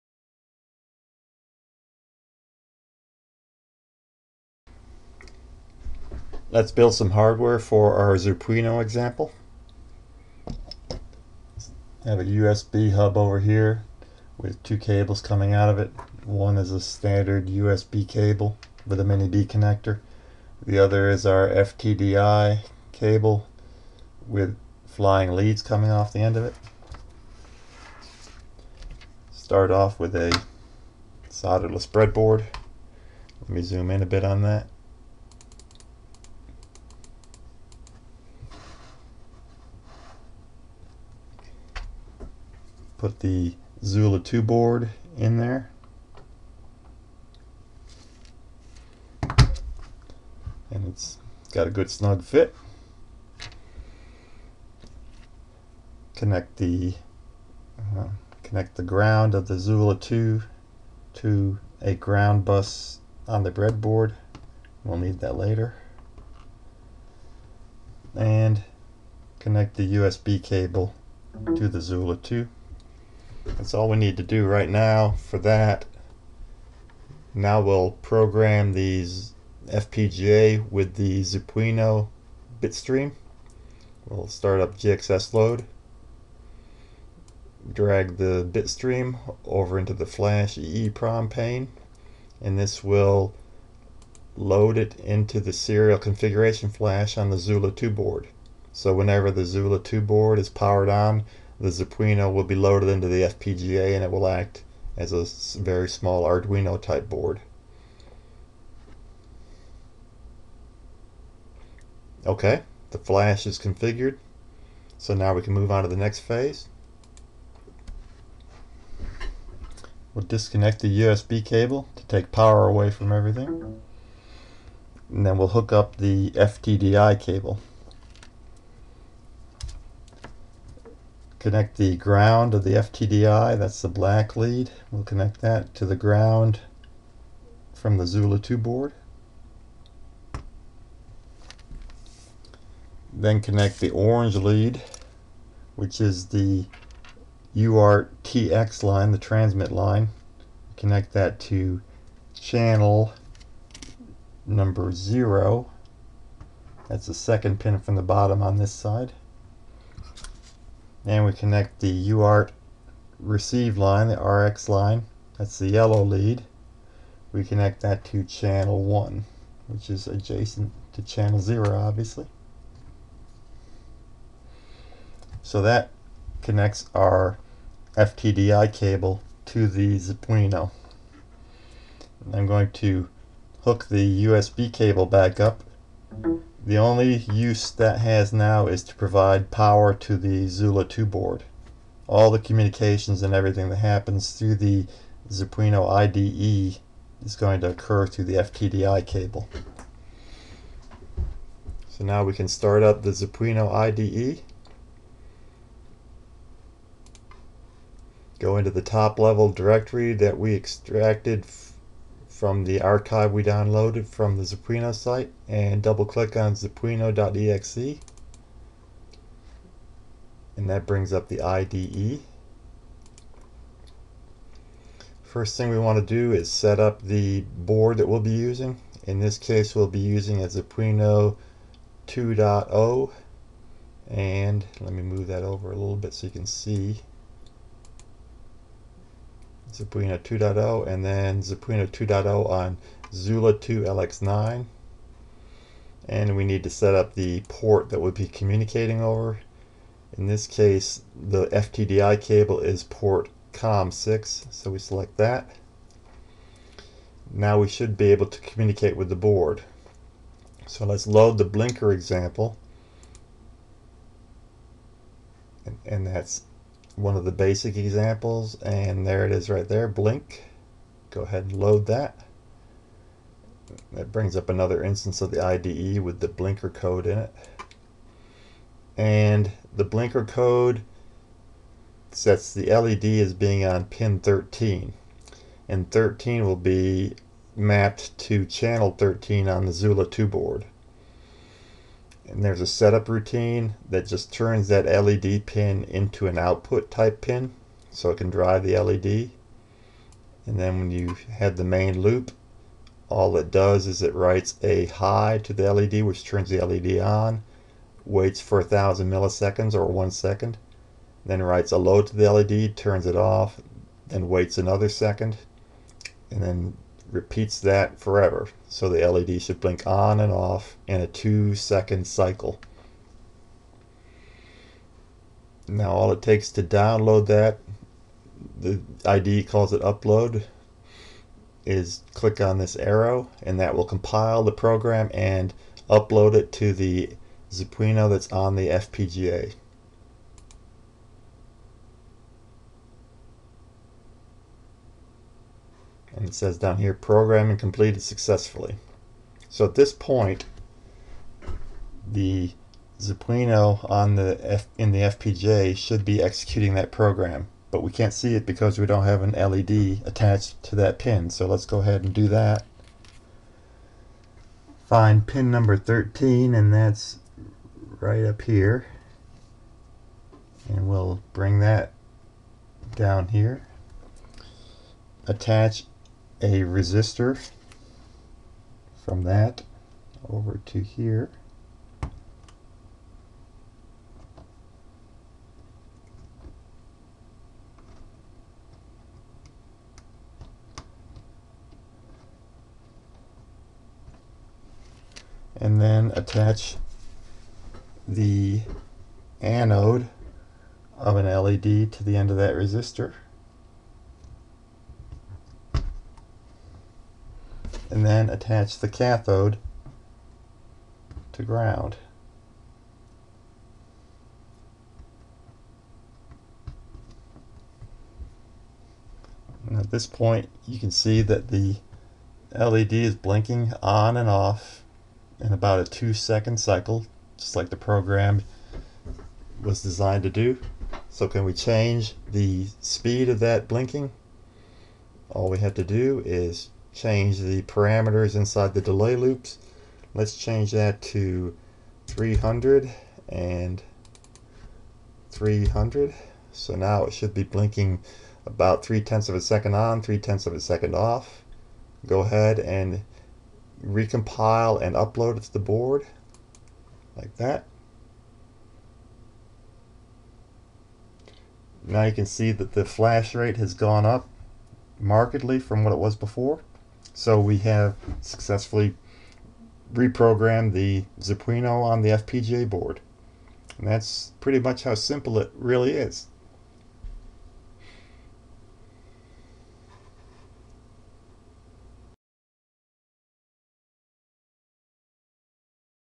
ZPUino#1
First example of running the ZPUino 32-bit processor on a XuLA2 FPGA board.